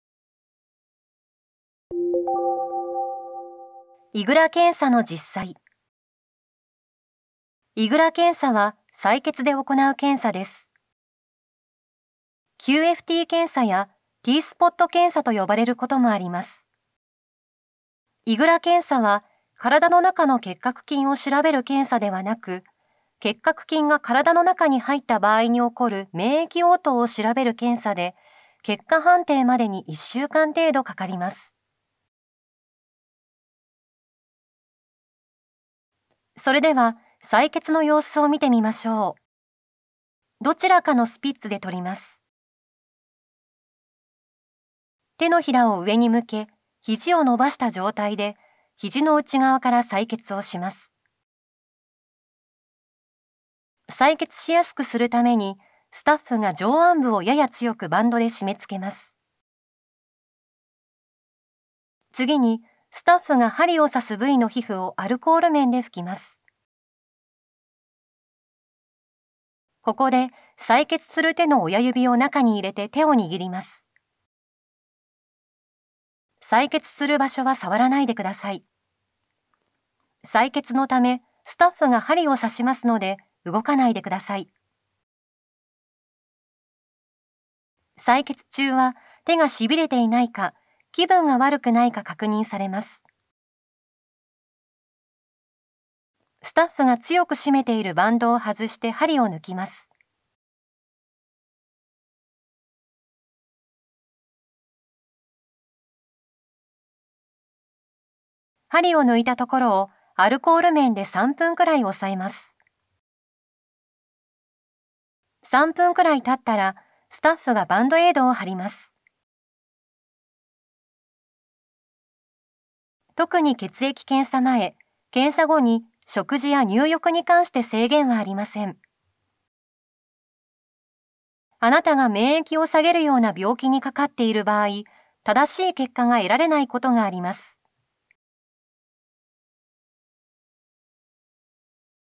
2-4 [Japanese] IGRA(血液)検査の実際
「2-4_IGRA(血液)検査の実際」長引くその咳結核かも The prolonged cough might be caused by tuberuculosis.この映像は、結核と潜在性結核感染症の検査、治療、保健所の支援について日本語で解説している、東京都が制作した動画です。